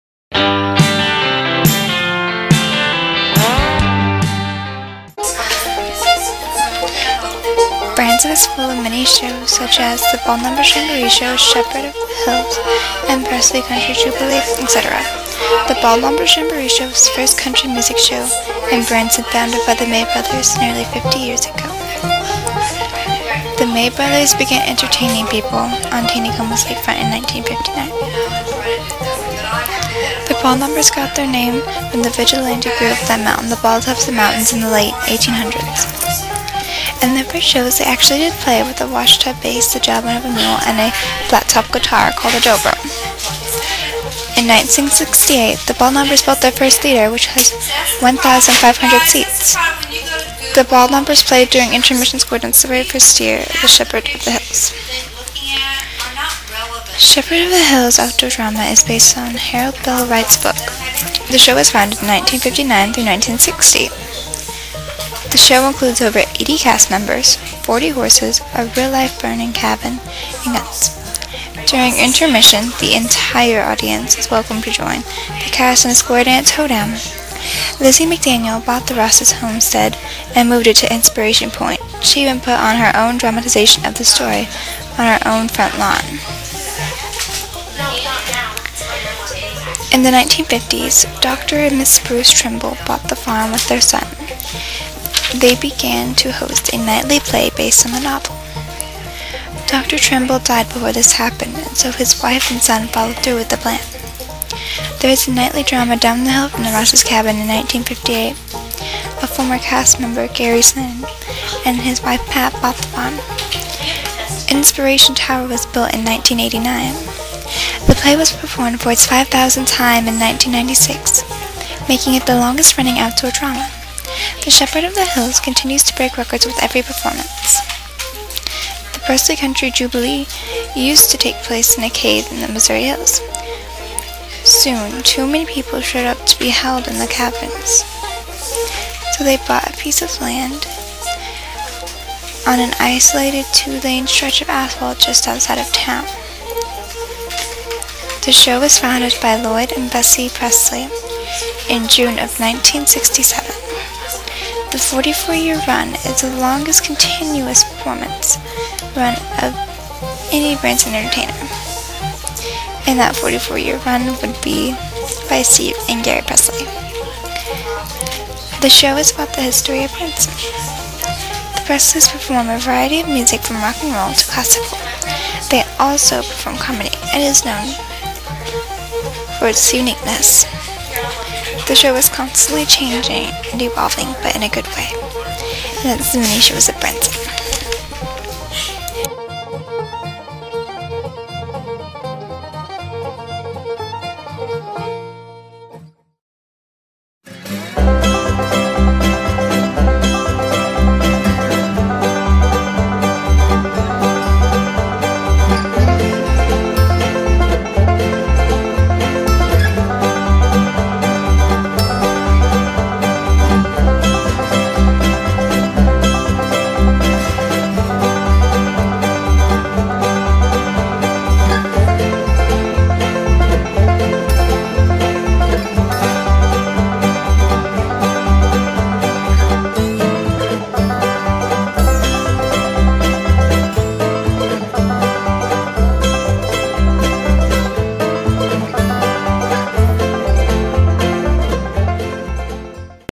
The Shows in Branson, Missouri
This student-created digital story is the result of an 8th grade Ozarks Studies research project at Forsyth Middle School. There may be inaccuracies of information or errors in documenting sources of information and images used.